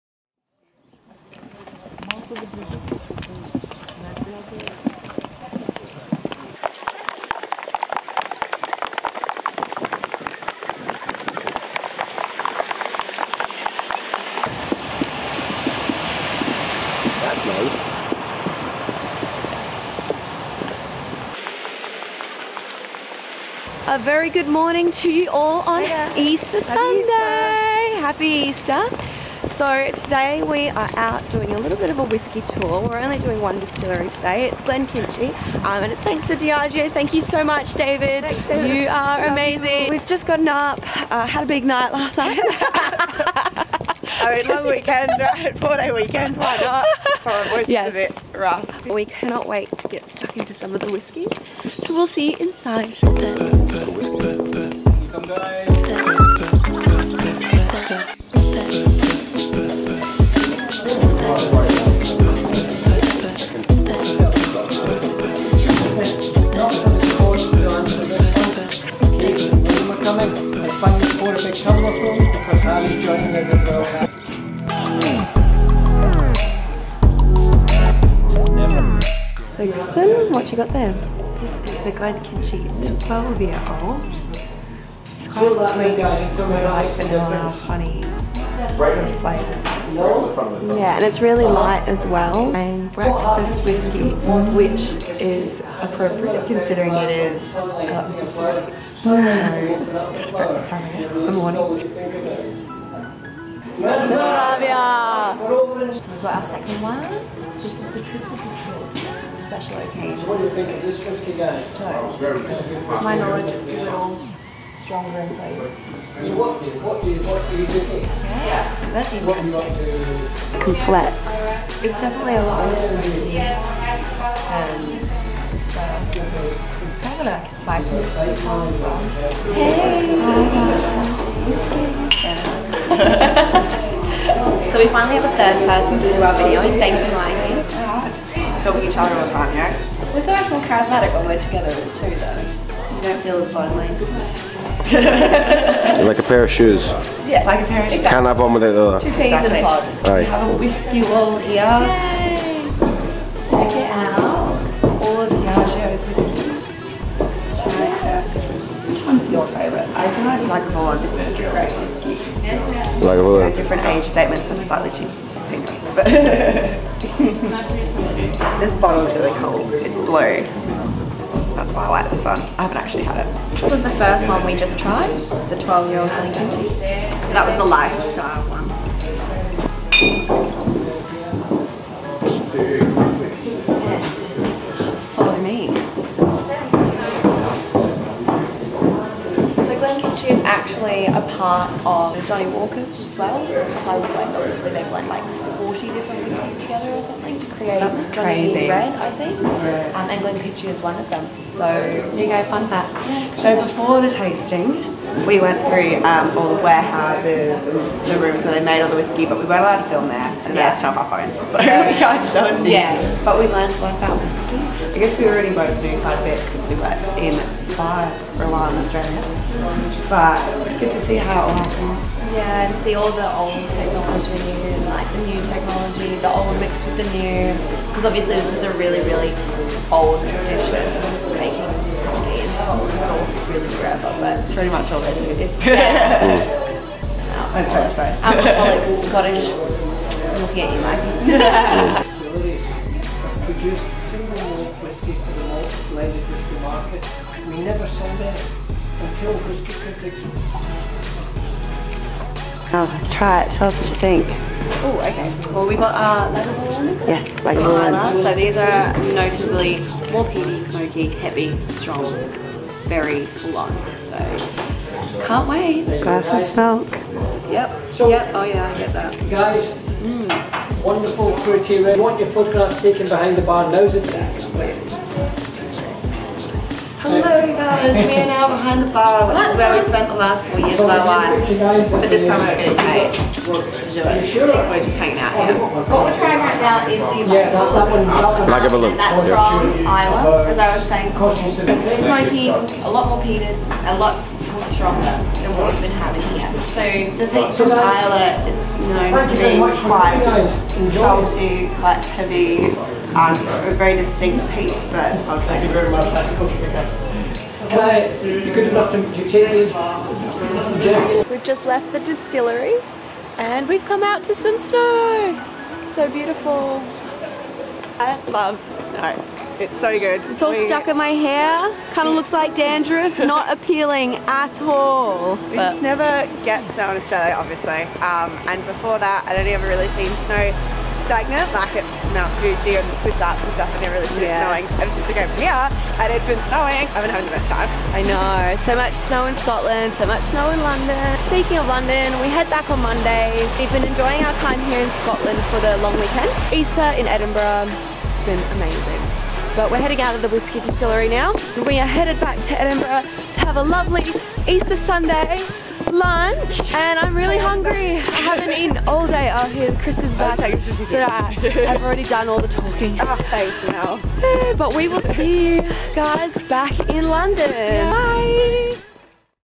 WHISKY TASTING IN SCOTLAND | GLENKINCHIE AND DIAGEO | THE ADELAIDIAN
Over the Easter long weekend, our wonderful friend David Sinclair of Diageo invited us to visit Glenkinchie Distillery, located just a stone's throw away from Edinurgh's city centre.

We were taken on an informative tour of the distillery, getting the chance to see some of the behind-the-scenes action, followed by our favourite part of the day: the whisky tasting, of course!

Forget the stigma that whisky belongs to old English men in Peaky Blinders-type caps, cigars in hand... These days whisky is for everyone, even for girls in their early 20's (such as ourselves)!